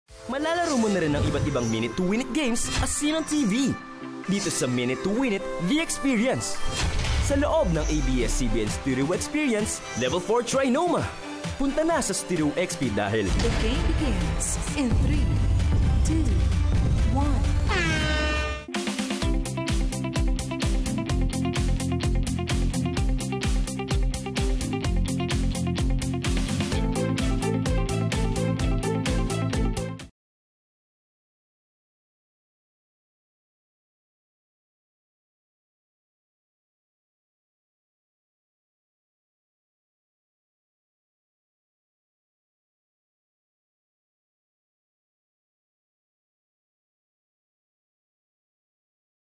Minute To Win It: The Experience at ABS-CBN Studio Experience
Try the Minute To Win It: The Experience inside the ABS-CBN Studio Experience, Level 4, TriNoma!